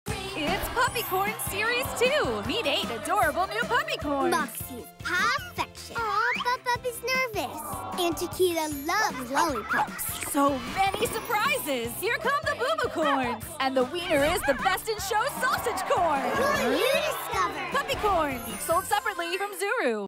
The sweetest new Series 2 Puppycorns have arrived including the cuddly Rare Sausage Corn.
There are 8 NEW Puppycorns to collect including the Rare Best-in Show Blue Sausage Dog! Each Puppycorns has a unique Peel 'n' Reveal heart which shows what your Puppycorn loves! Unbox 7 layers of surprises inside, including new Boo-Boocorn Puppies, Puppycorn Sticker Pack, and new Puppycorn magical ooey gooey slime poop! Which Puppycorn will be your new furry best friend?!